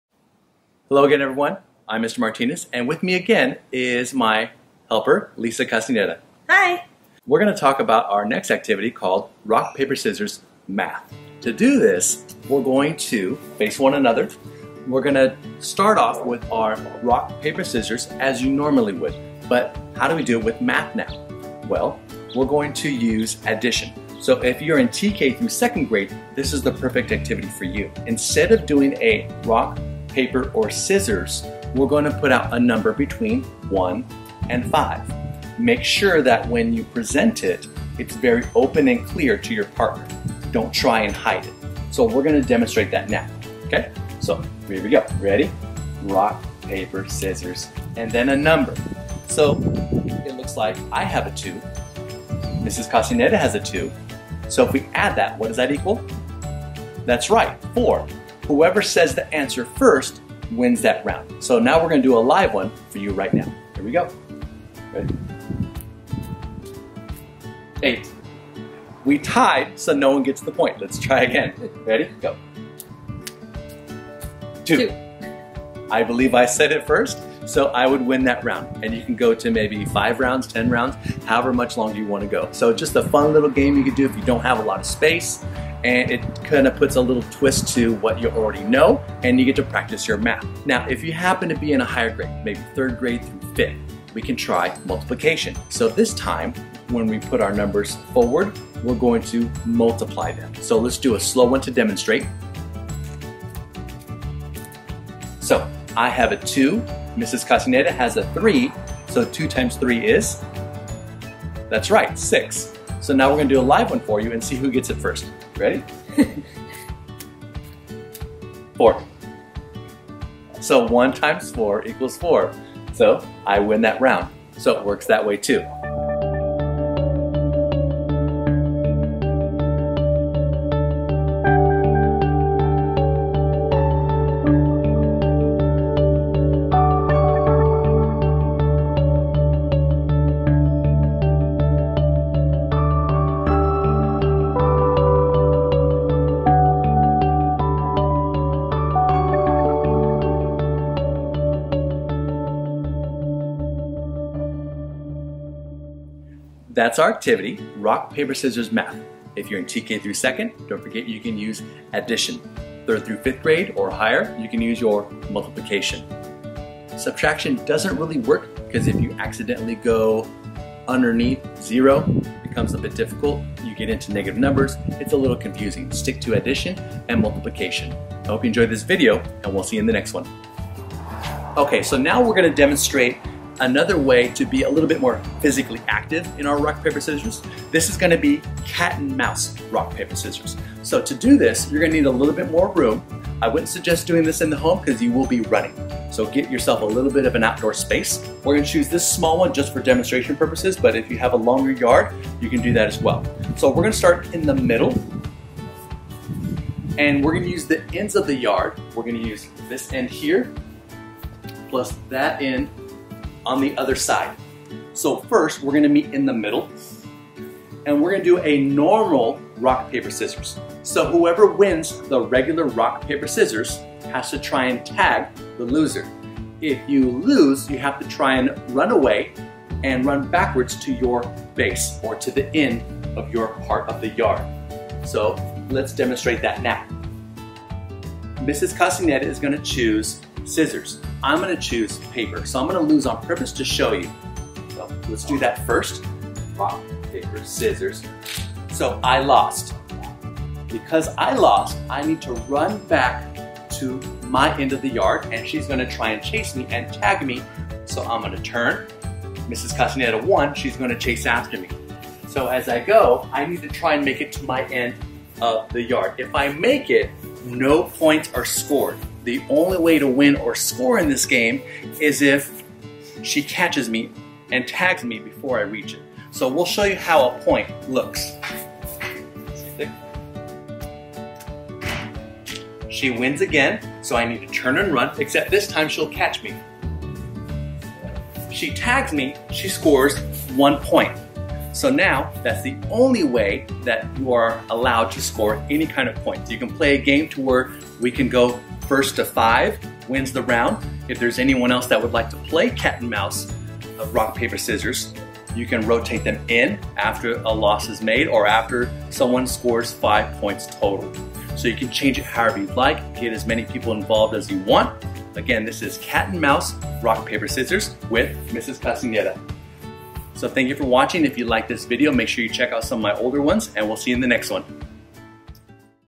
Rock, Paper, Scissors, Math! - LUSD PE
For today's video, we'll use a classic game of Rock, Paper, Scissors and add in some Math! There's also a bonus game at the end. Enjoy!

TK - 2nd Grade: Addition
3rd - 5th Grade: Multiplication